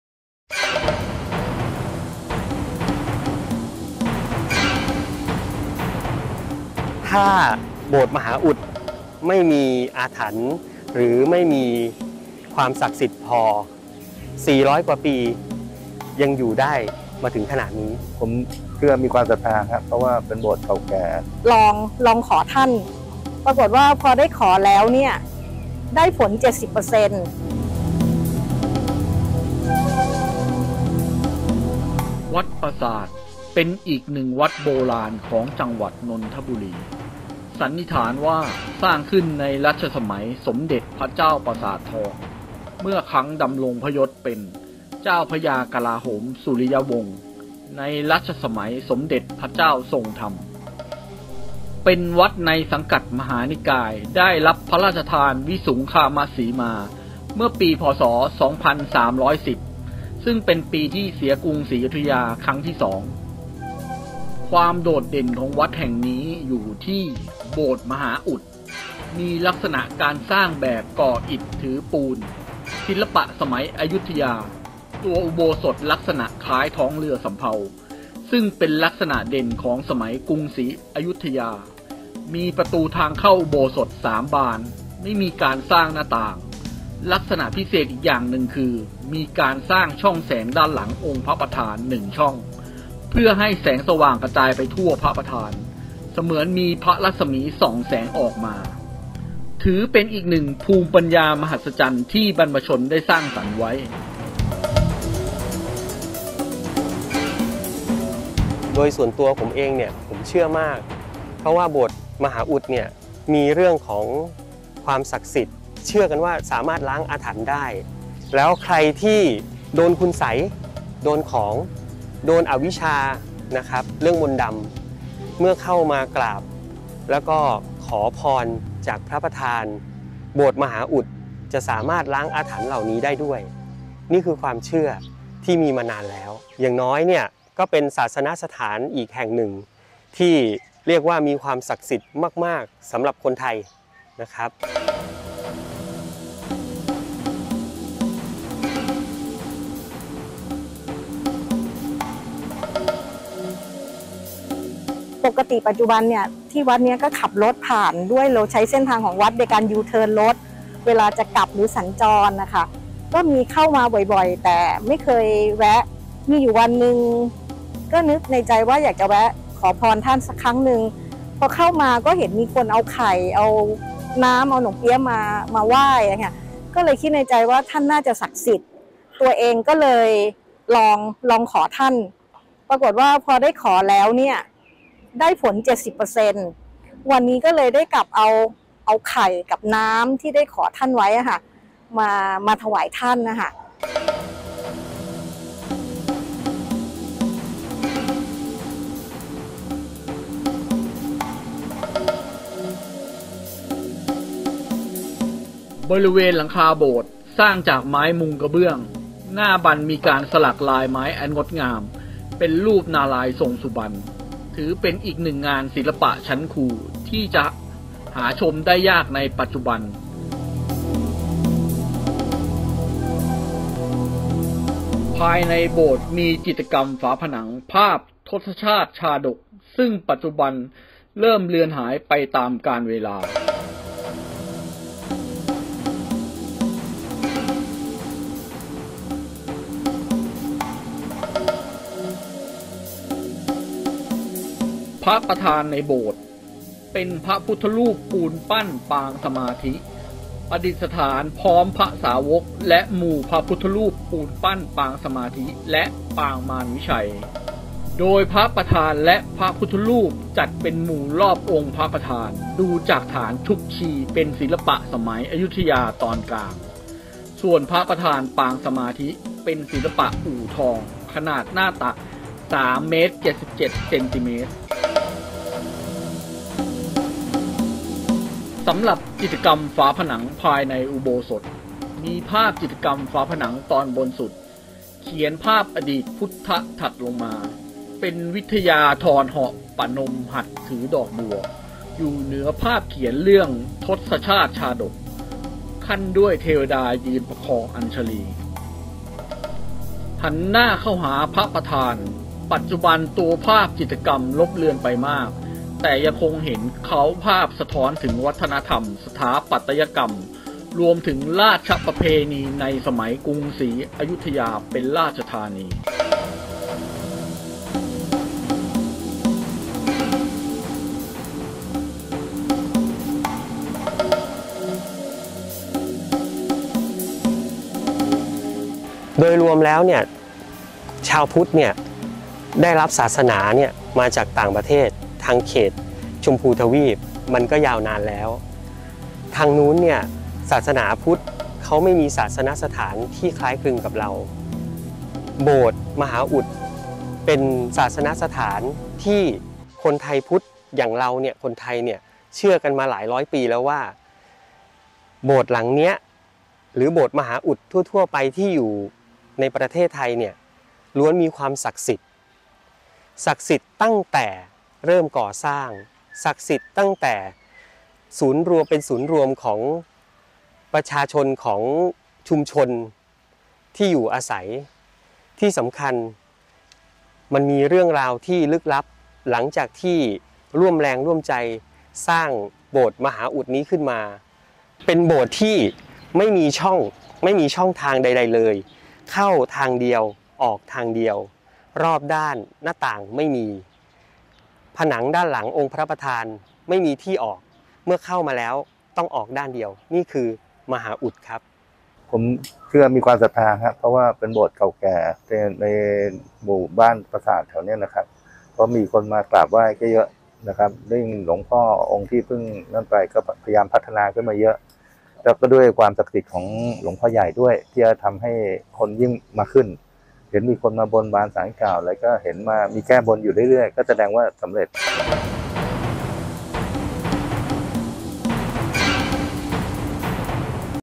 โบสถ์มหาอุตม์ ตามความเชื่อ ความศรัทธา ของชาวบ้านและผู้ที่มากราบไหว้
โบสถ์มหาอุตม์ อายุกว่า 400 ปี ตามความเชื่อของโบราณเป็นที่ทำพิธีกรรมอันศักดิ์สิทธิ์  วัดปราสาท เป็นอีกหนึ่งวัดโบราณของจังหวัดนนทบุรี
สันนิษฐานว่าสร้างขึ้นในรัชสมัยสมเด็จพระเจ้าปราสาททอง
ได้รับพระราชทานวิสุงคามสีมาเมื่อปี พ.ศ.2310
ซึ่งเป็นปีที่เสียกรุงศรีอยุธยา ครั้งที่ 2